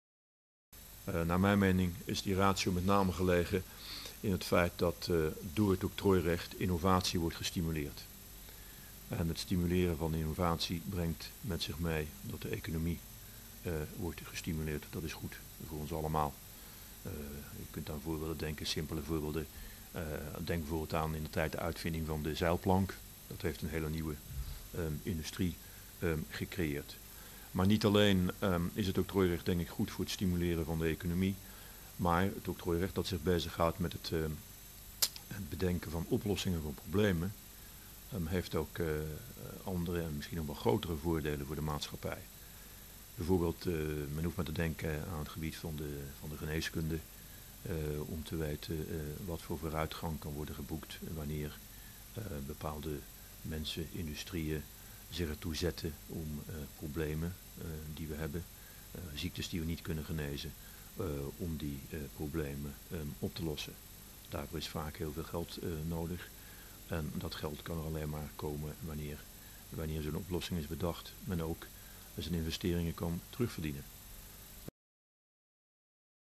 RIB-F-041010 - WH0102 - de ratio van het octrooirecht
Wat is de ratio van het octrooirecht. Prof. Willem Hoyng wijst op het belang voor de samenleving van de incentive functie die er van octrooien uitgaat.